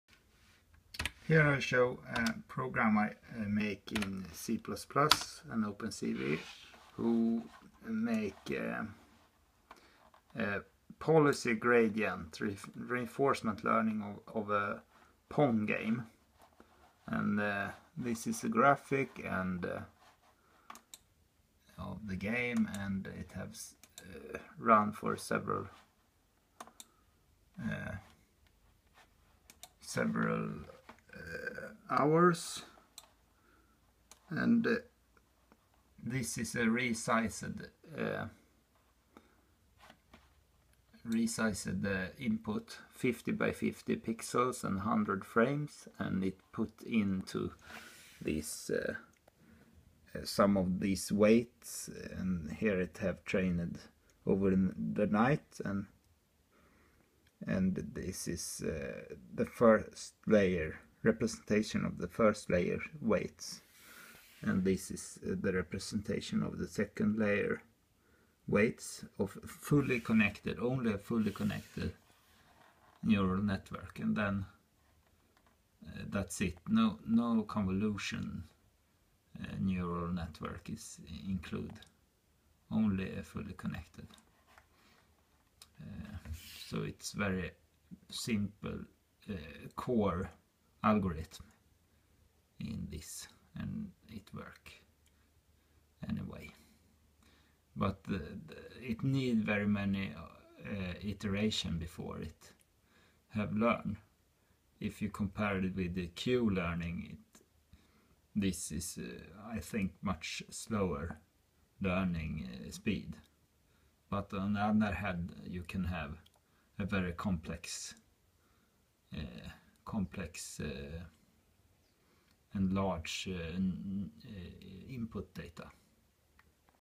Policy gradient reinforcement learning pong raspberry pi
Written in C++ and OpenCV library.

Reinforcement Machine Learning the Agent feeded by 50x50 pixels and 100 frames each episode. The actions is UP/DOWN.